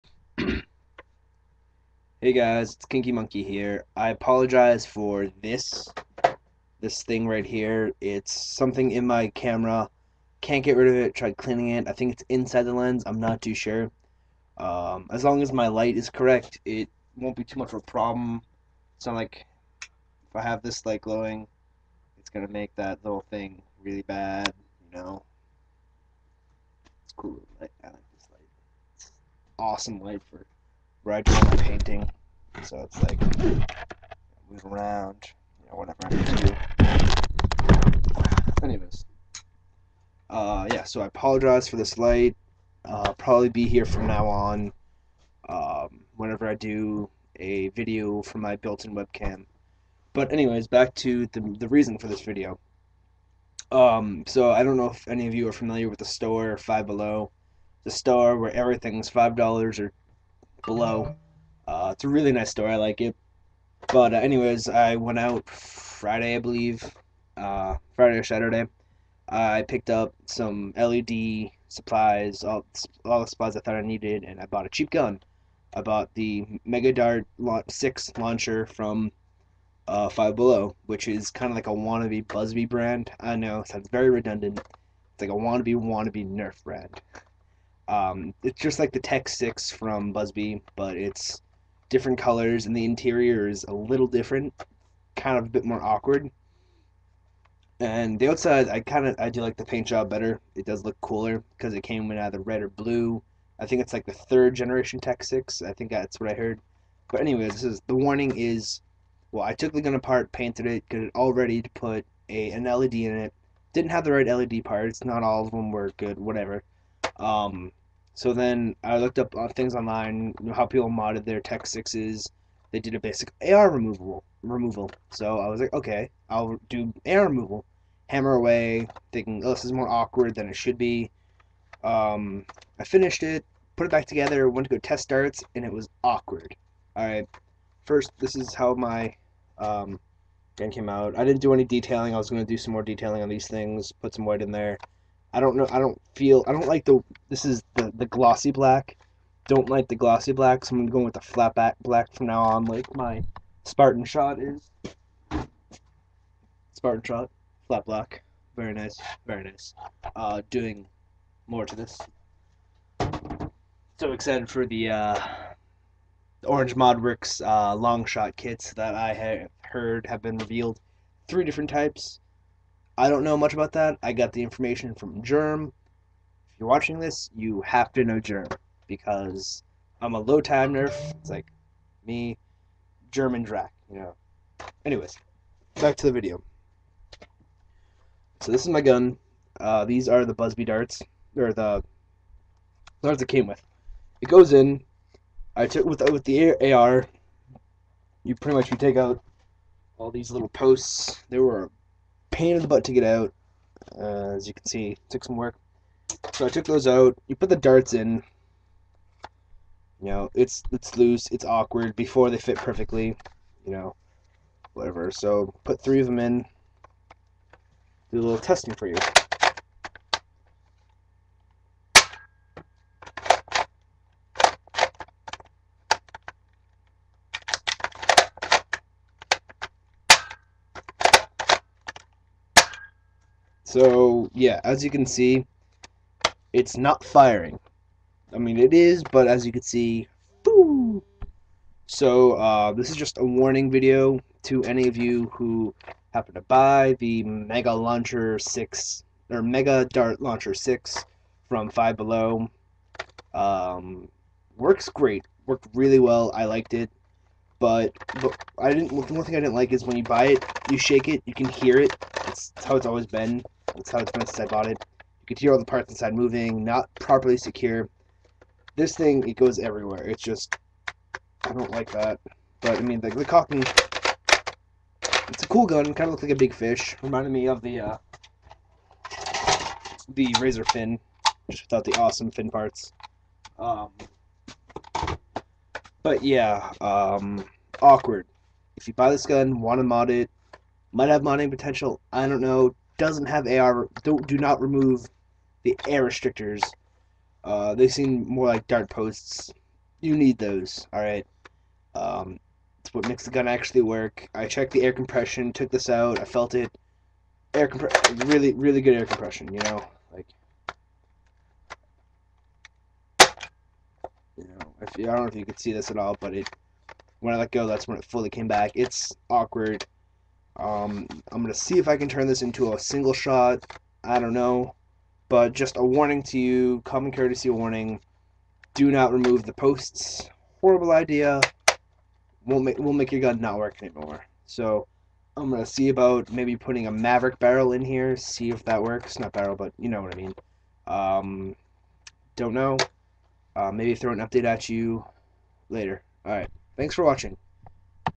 DO NOT REMOVE AR FROM "mega dart 6 launcher"
i bought i mega dart 6 launcher from 5 below. it is a knock off of the tek 6 from buzz bee. i took out the posts just like you can in the tek 6 and it stoped the gun from fireing. just a warning to you all.